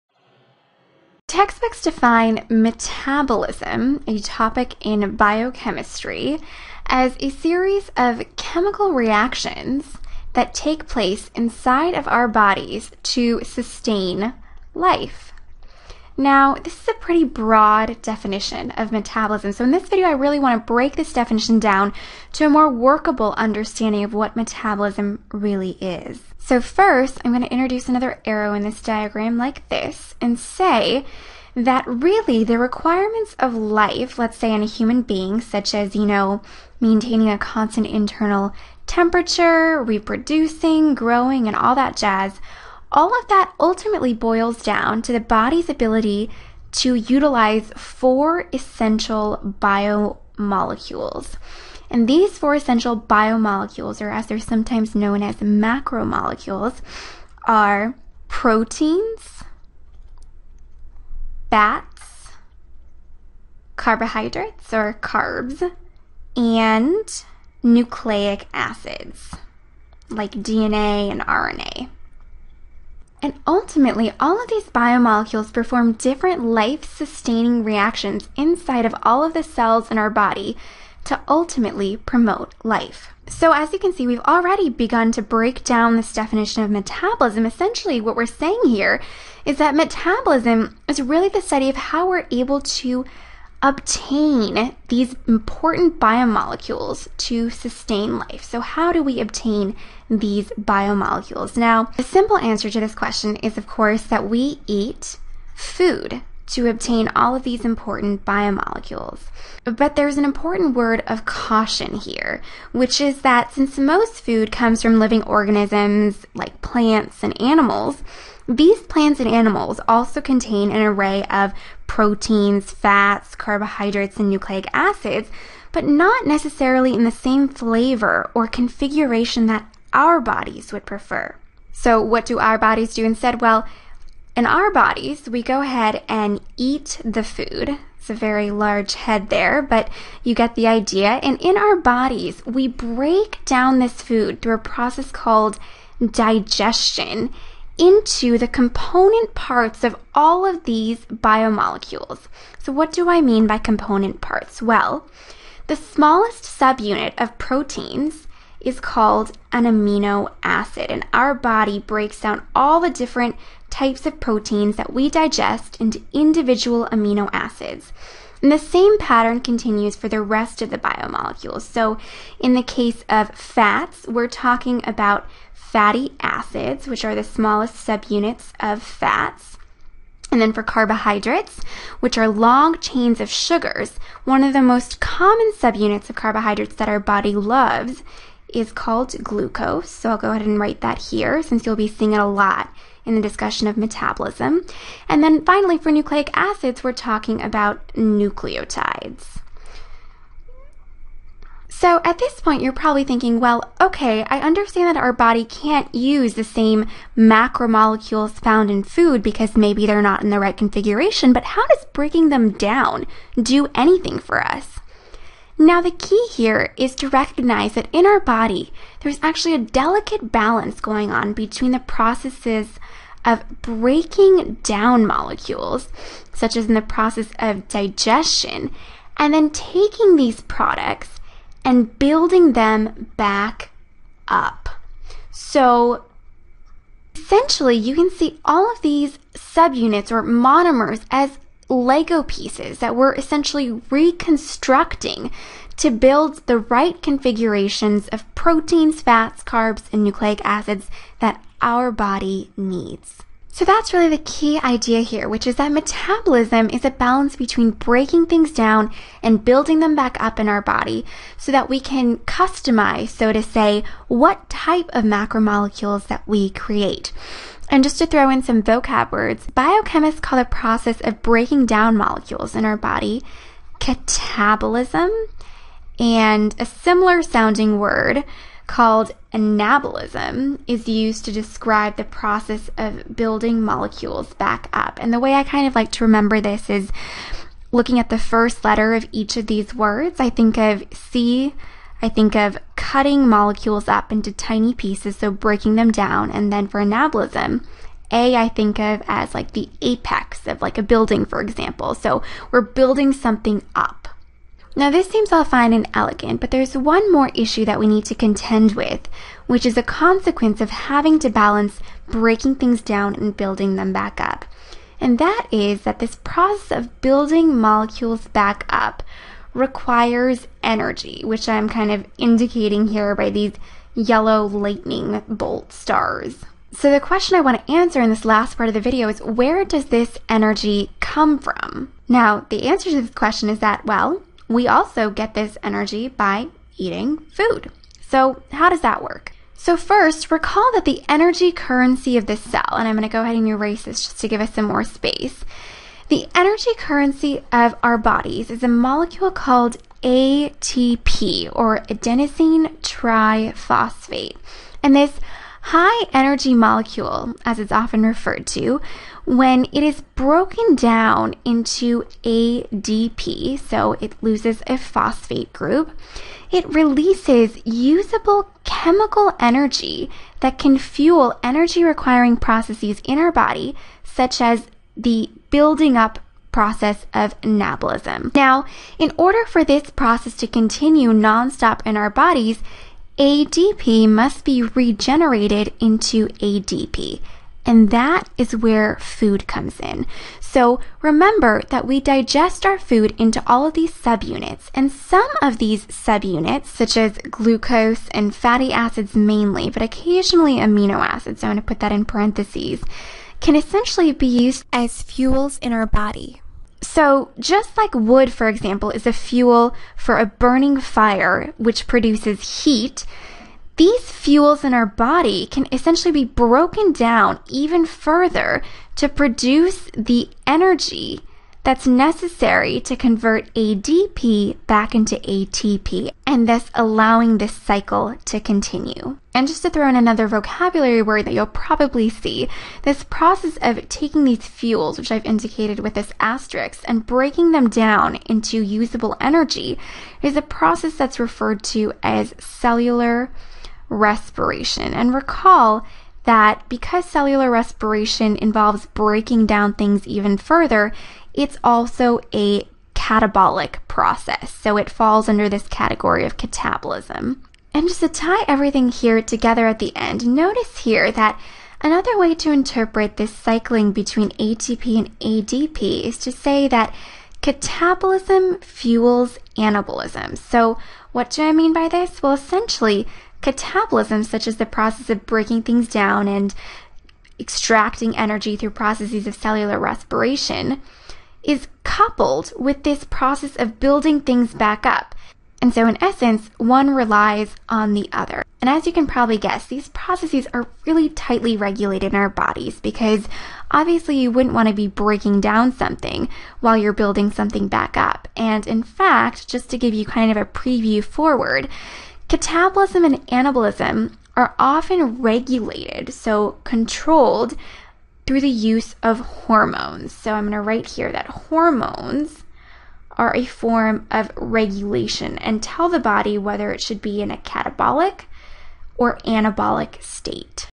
Overview of metabolism: Anabolism and catabolism | Biomolecules | MCAT | Khan Academy
What is the purpose of metabolism? Learn about the two major divisions in metabolism: anabolism (building up) and catabolism (breaking down).  By Jasmine Rana. Created by Jasmine Rana.